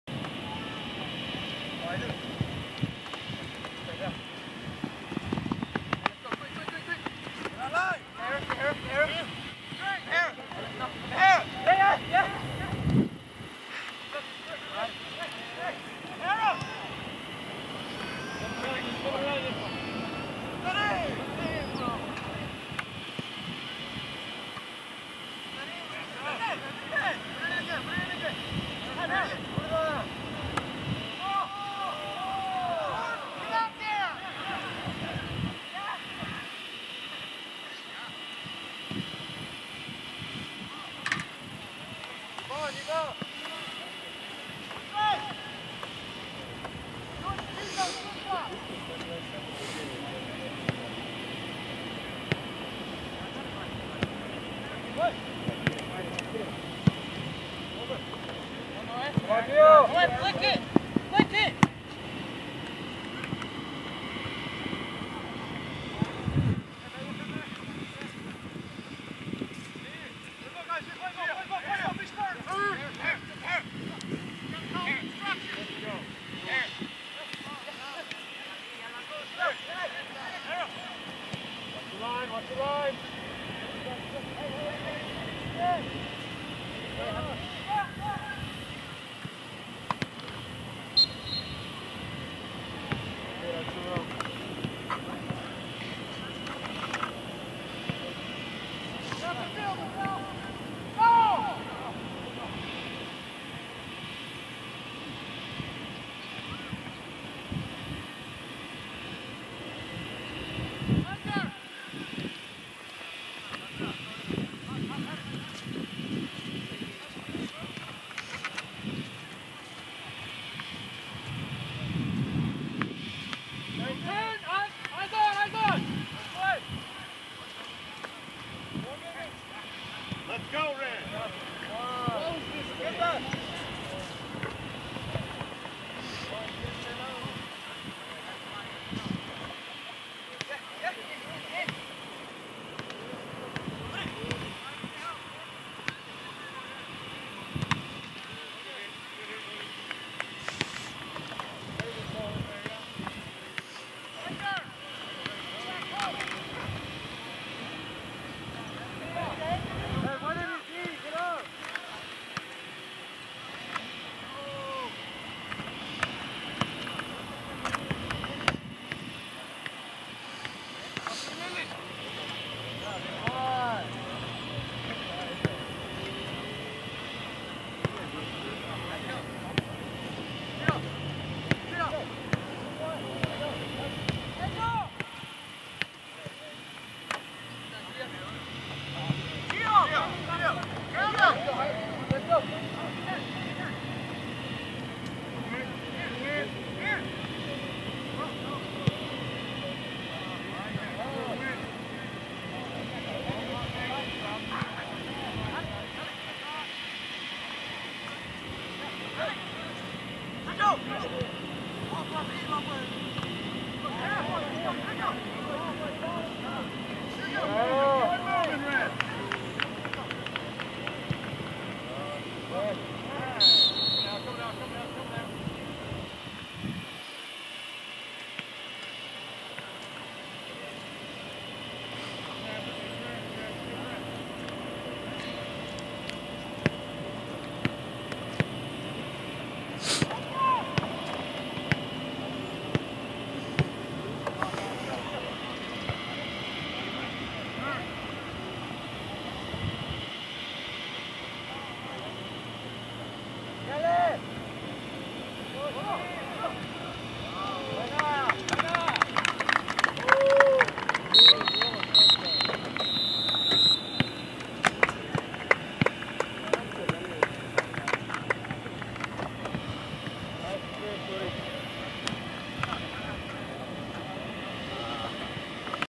Wichita North vs Wichita West, Gentlemen's Soccer, 10-10-2020, Part 4
Wichita North 10 (7-3)
Wichita West 0